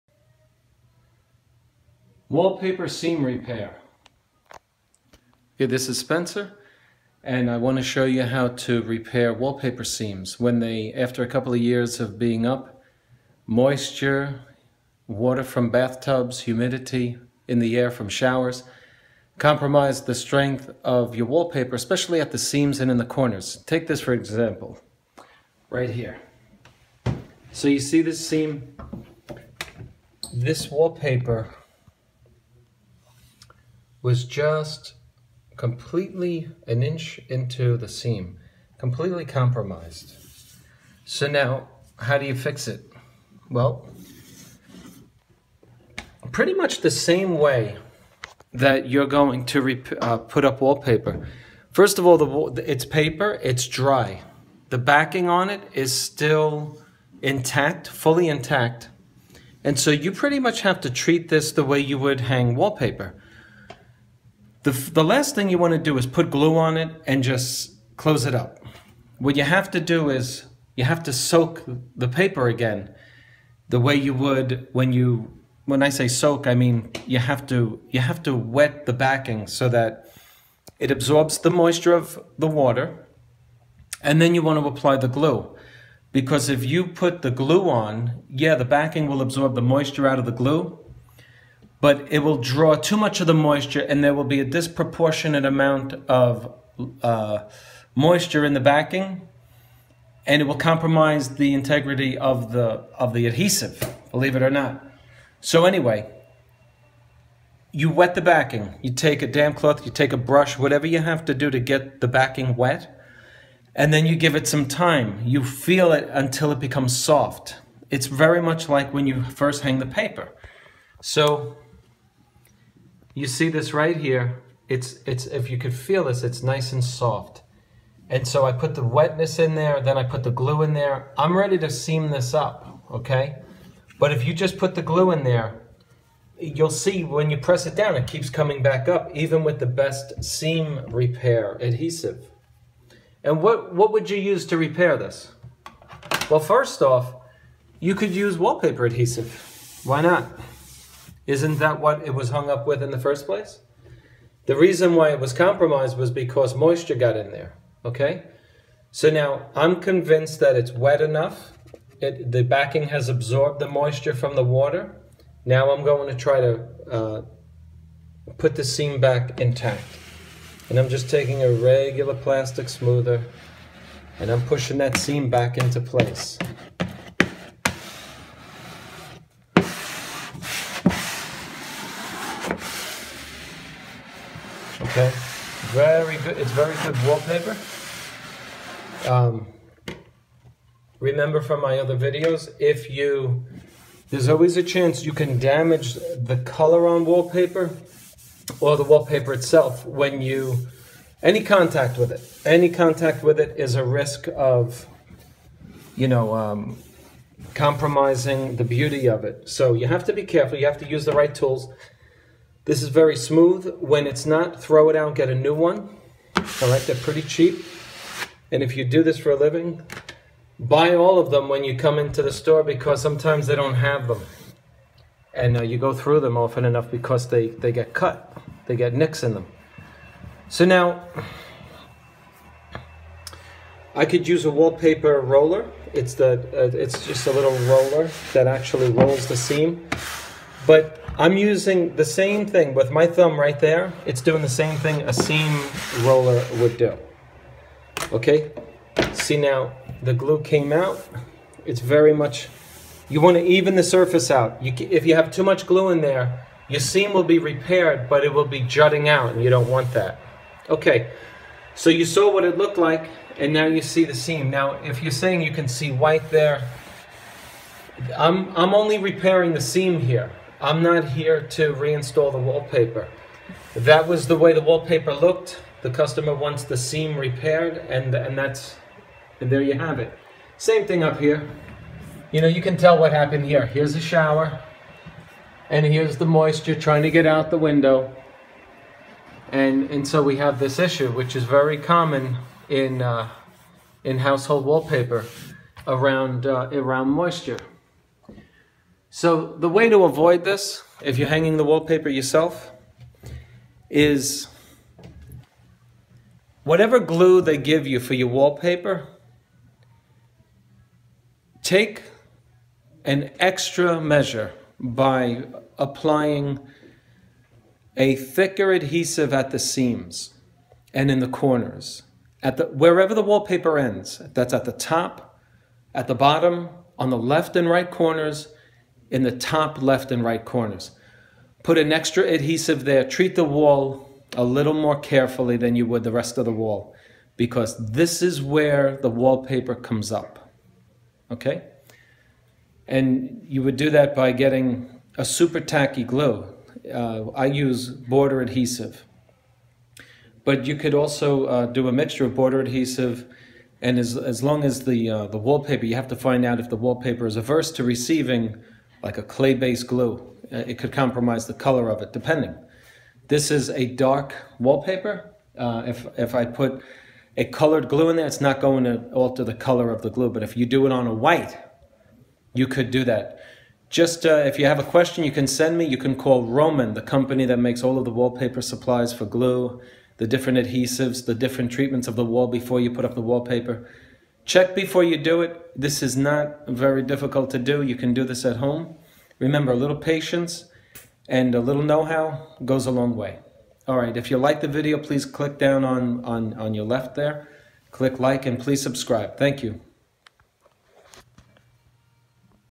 How To Repair Wallpaper Seams Part I - Spencer Colgan
In this video, I show you how to repair a common wallpaper seam using a specially mixed adhesive to prevent recurrence.